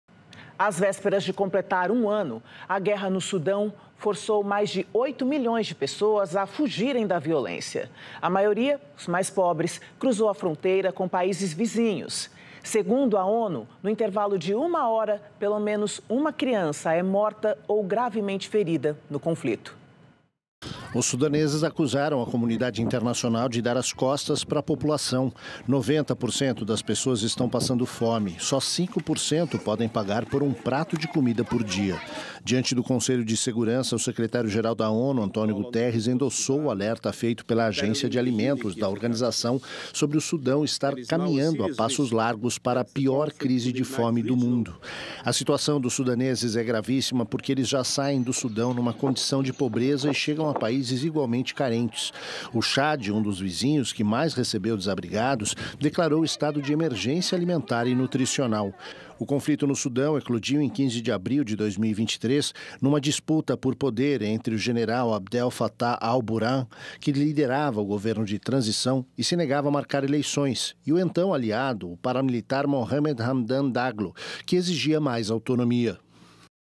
Guerra no Sudão força mais de 8 milhões de pessoas a fugirem da violência
Às vésperas de completar um ano, a guerra no Sudão forçou mais de 8 milhões de pessoas a fugirem da violência. A maioria, os mais pobres, cruzou a fronteira com países vizinhos.

Diante do Conselho de Segurança, o secretário-geral da ONU, António Guterres, endossou o alerta feito pela agência de alimentos da organização sobre o Sudão estar caminhando a passos largos para a "pior crise de fome do mundo”.

Os sudaneses acusaram a comunidade internacional de dar as costas para a população.90% das pessoas estão passando fome. Só 5% pode pagar por um prato de comida por dia.